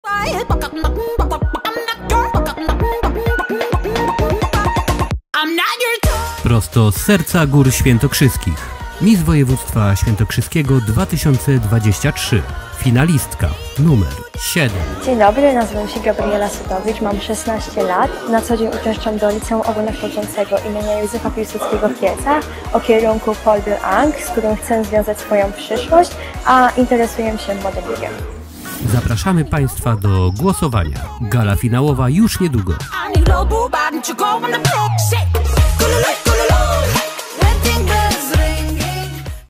Nr 7 Gabriela Sutowicz FINALISTKA Miss Województwa Świętokrzyskiego 2023
Akademia makijażu Ewy Filipeckiej
Miejsce: Hotel Uroczysko Cedzyna koło Kielc